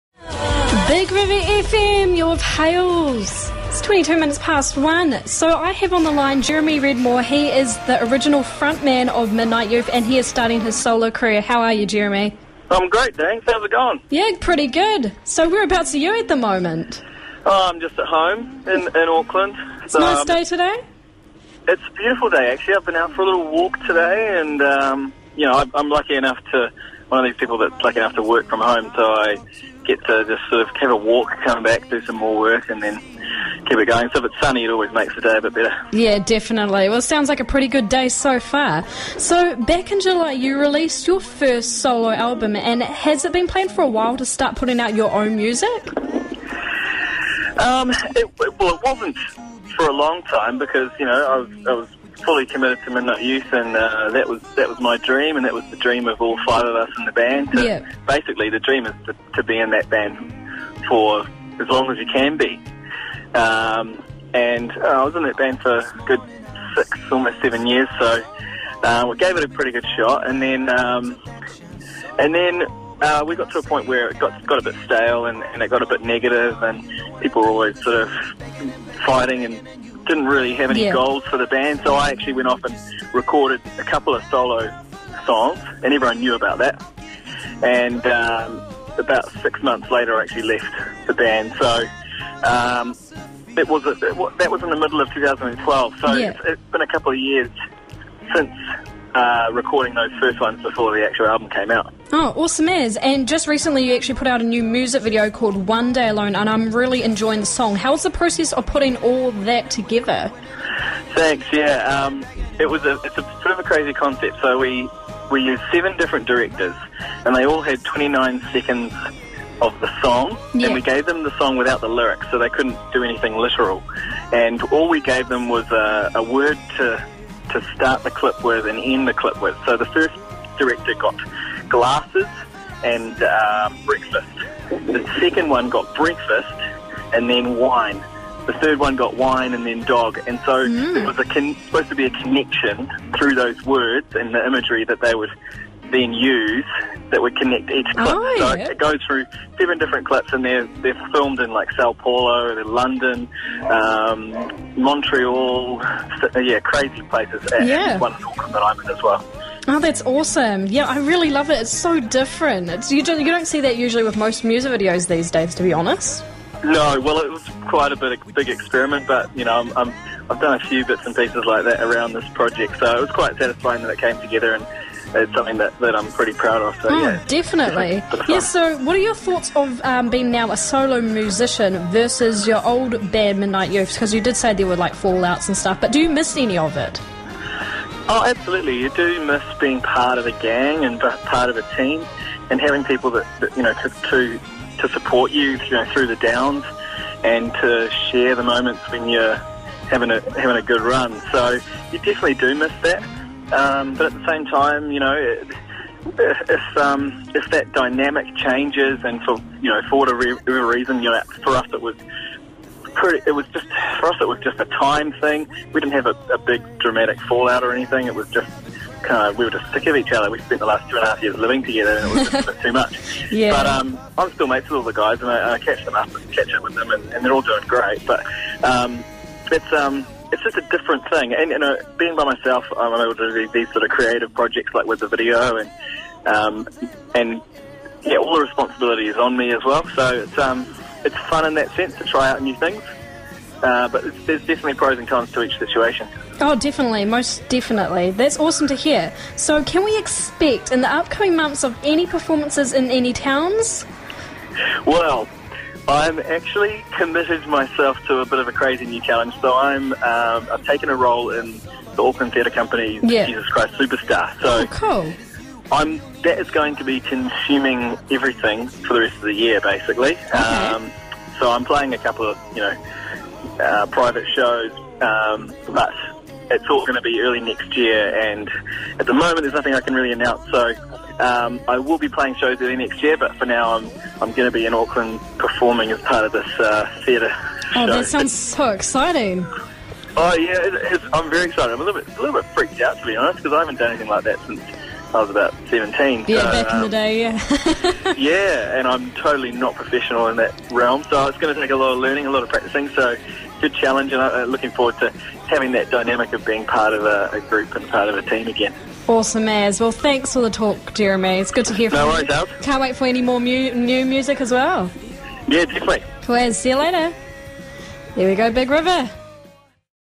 Interview with Jeremy Redmore
It was a pleasure to talk to ex-Midnight Youth frontman, Jeremy Redmore yesterday. So talented! Check out his channel below: